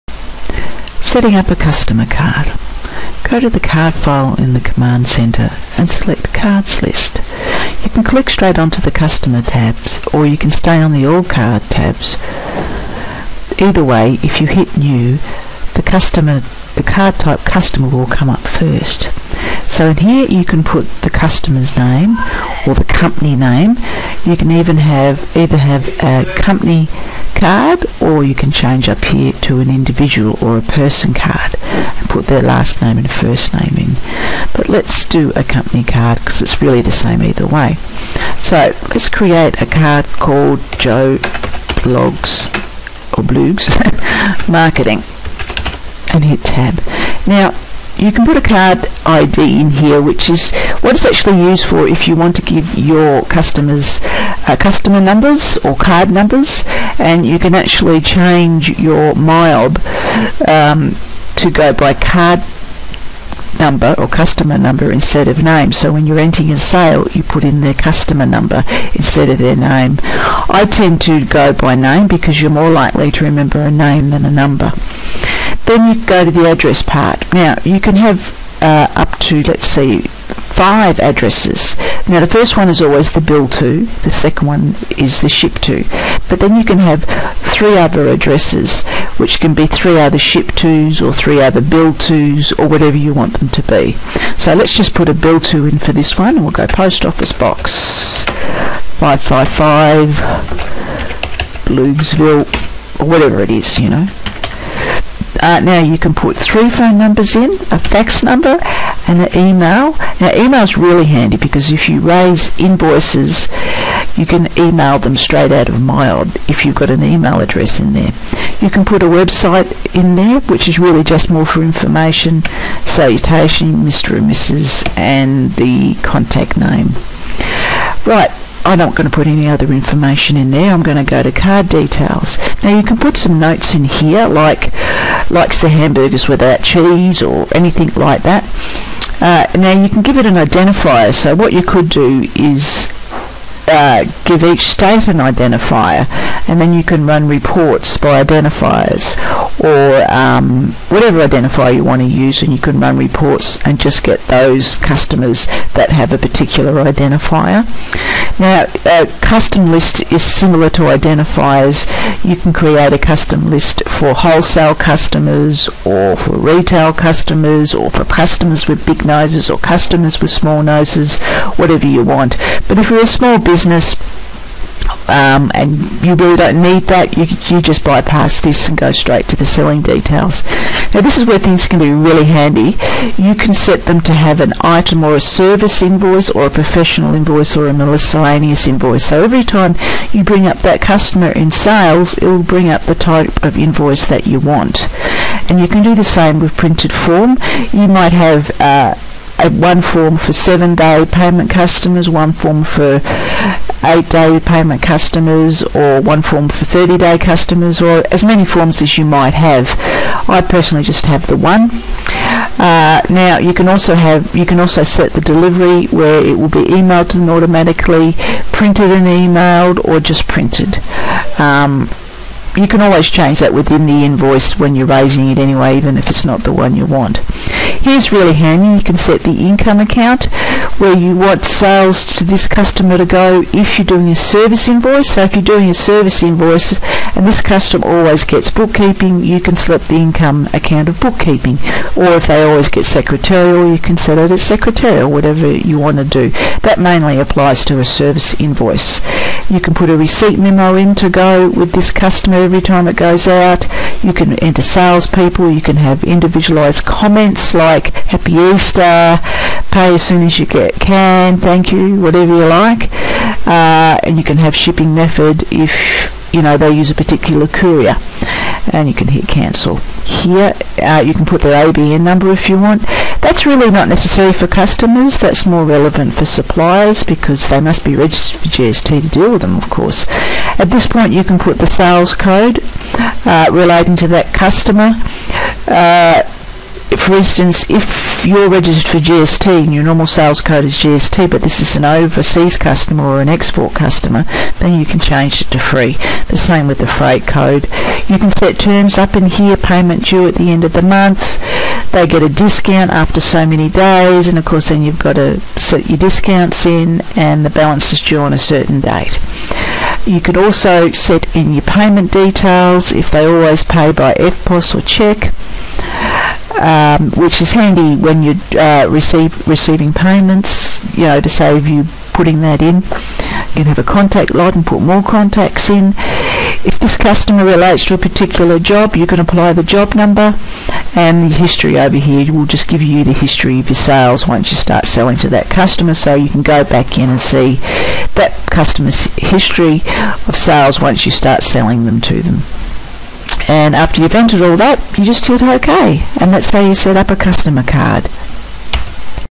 Setting up a customer card in MYOB
This is a video on how to best set up your customer cards to get the most out of them